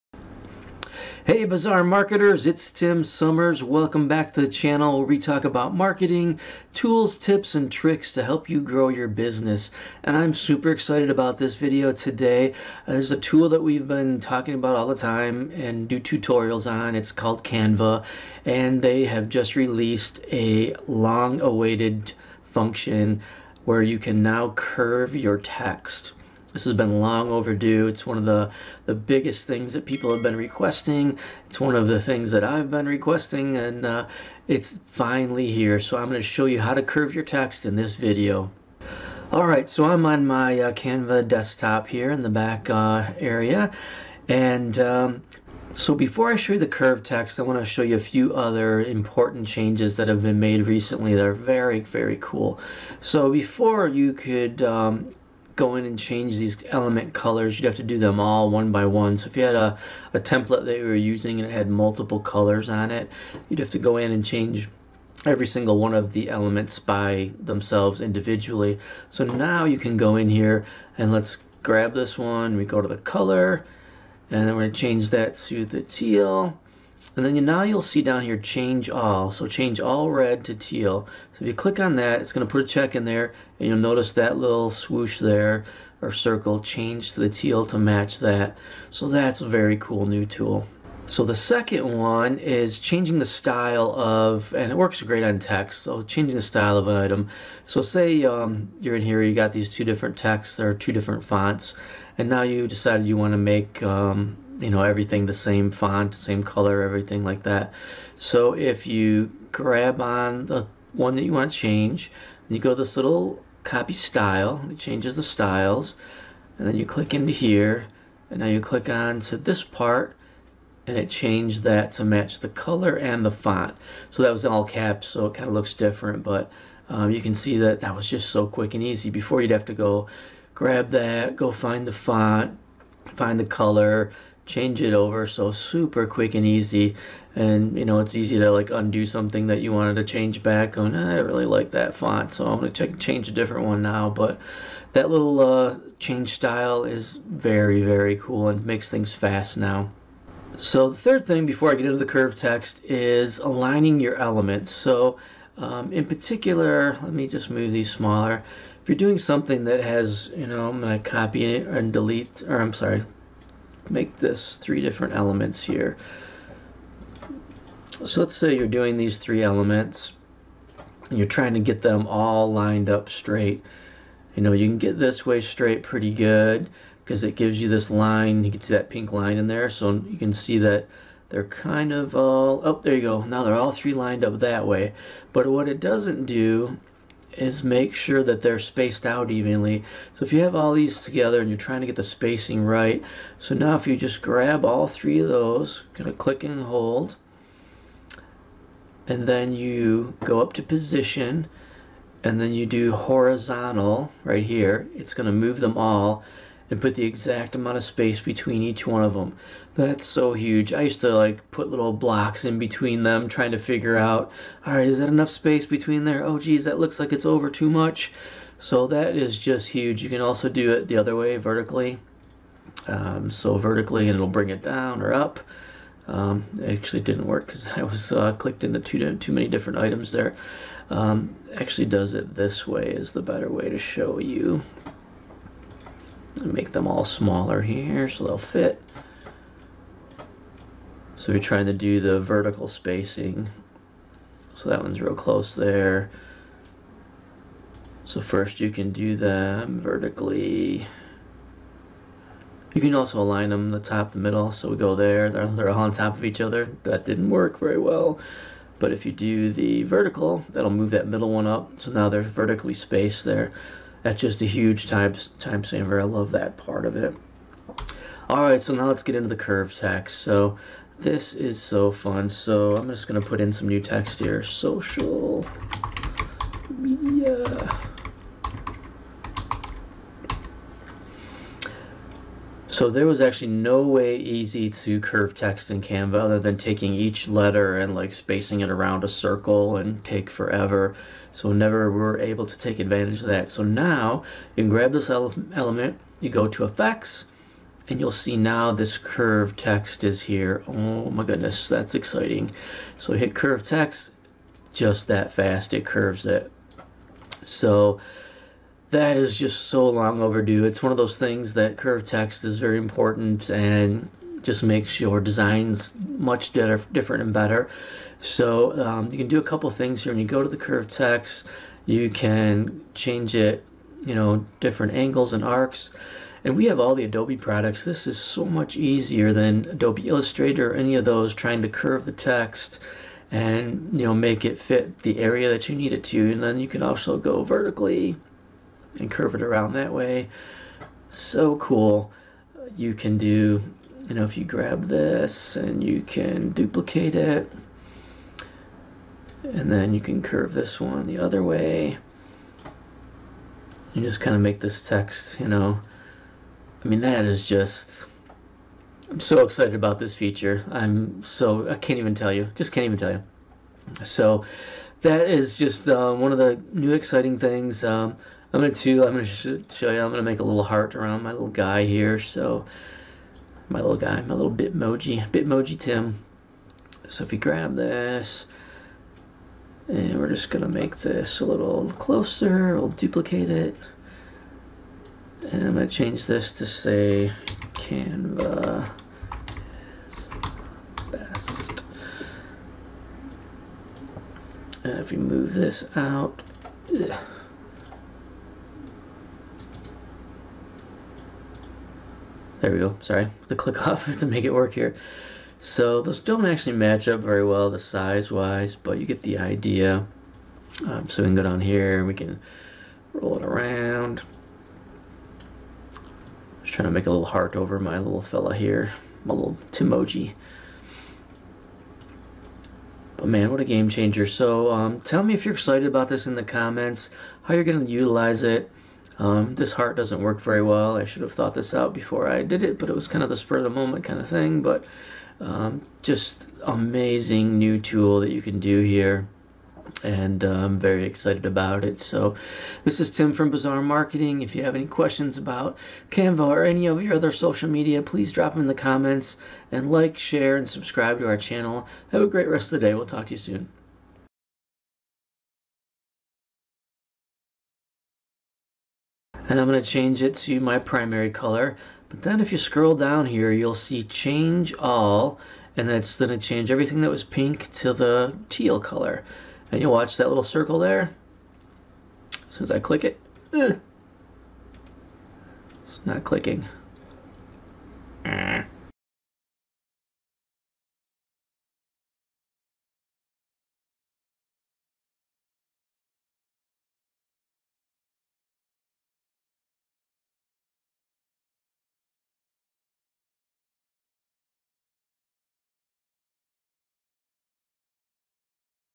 How To CURVE Text In Canva | Canva Tutorial | NEW 2020 Feature
It's been a long-overdue feature to the amazing Canva platform - Curved Text. In this Canva Tutorial, I break down how easy it is to curve the text in your Social Media Marketing graphics.

Marketing should generate results.  Every business is different.  Cookie-cutter marketing strategies have no place here at Bizarre Marketing.